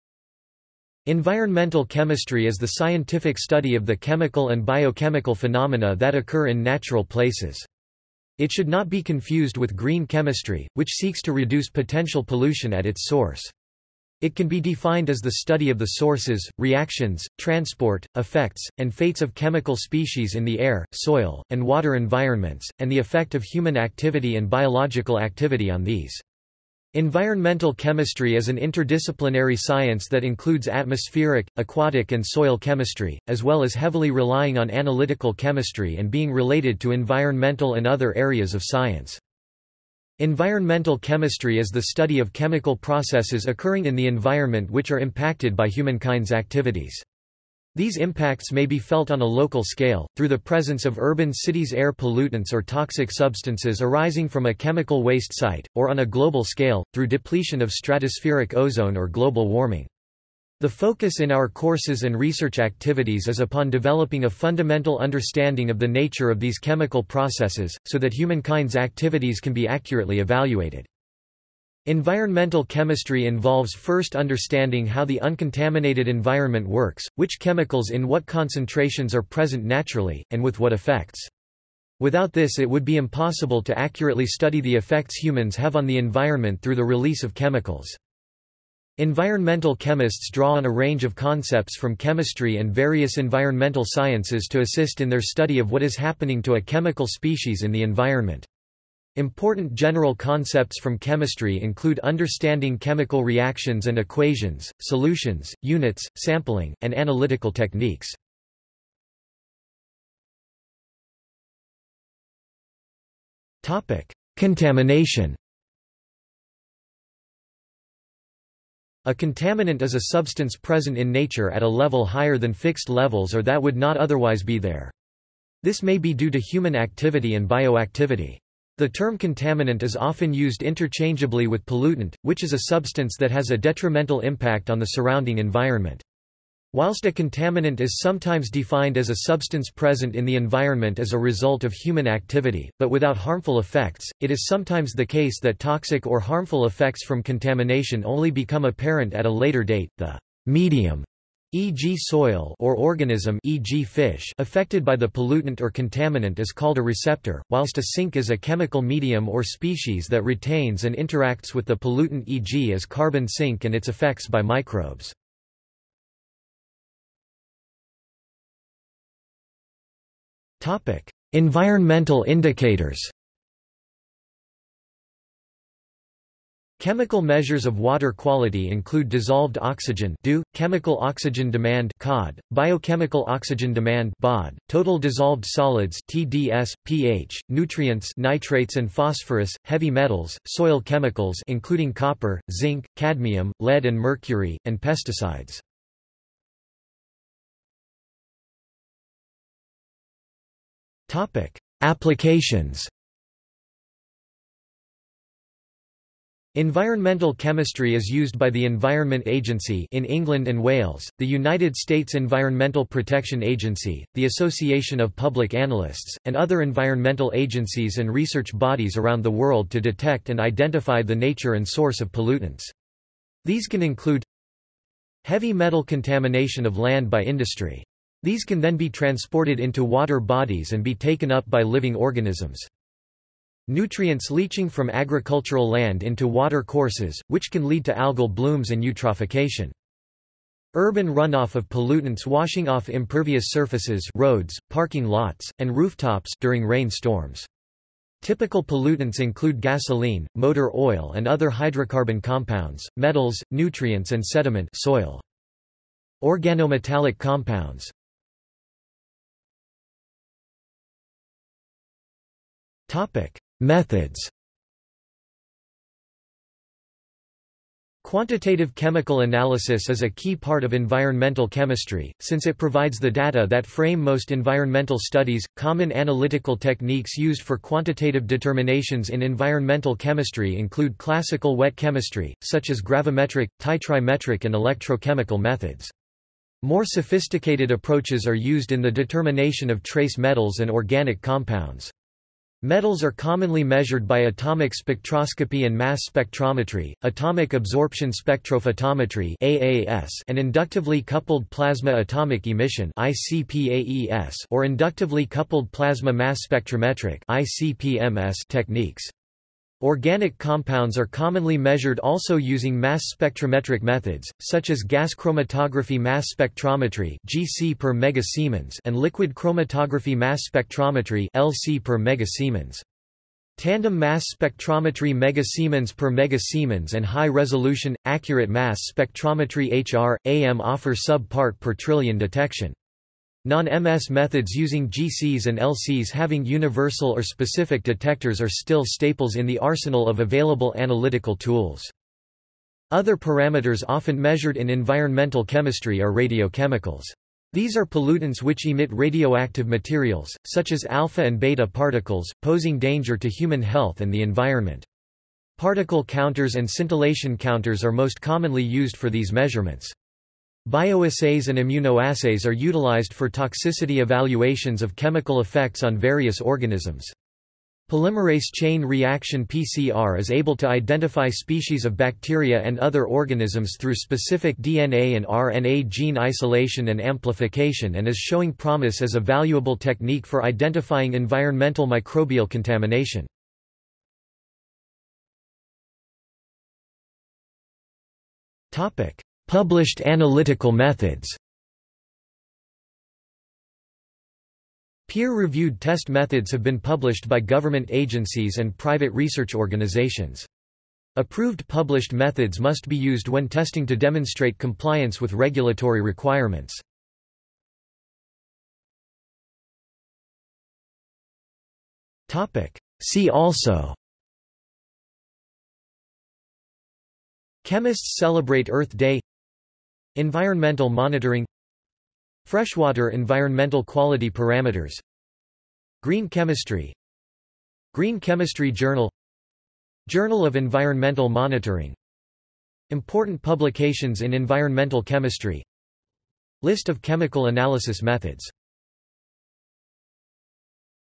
Environmental chemistry | Wikipedia audio article
00:01:50 1 Contamination
00:02:47 2 Environmental indicators
00:03:17 3 Applications
00:04:19 4 Methods
00:06:25 5 Published analytical methods
00:06:48 6 See also

- Socrates

SUMMARY
Environmental chemistry is the scientific study of the chemical and biochemical phenomena that occur in natural places. It should not be confused with green chemistry, which seeks to reduce potential pollution at its source. It can be defined as the study of the sources, reactions, transport, effects, and fates of chemical species in the air, soil, and water environments; and the effect of human activity and biological activity on these. Environmental chemistry is an interdisciplinary science that includes atmospheric, aquatic and soil chemistry, as well as heavily relying on analytical chemistry and being related to environmental and other areas of science.
Environmental chemistry is the study of chemical processes occurring in the environment which are impacted by humankind's activities. These impacts may be felt on a local scale, through the presence of urban cities' air pollutants or toxic substances arising from a chemical waste site, or on a global scale, through depletion of stratospheric ozone or global warming. The focus in our courses and research activities is upon developing a fundamental understanding of the nature of these chemical processes, so that humankind's activities can be accurately evaluated.
Environmental chemistry involves first understanding how the uncontaminated environment works, which chemicals in what concentrations are present naturally, and with what effects. Without this it would be impossible to accurately study the effects humans have on the environment through the release of chemicals.
Environmental chemists draw on a range of concepts from chemistry and various environmental sciences to assist in their study of what is happening to a chemical species in the environment. Important general concepts from chemistry include understanding chemical reactions and equations, solutions, units, sampling, and analytical techniques.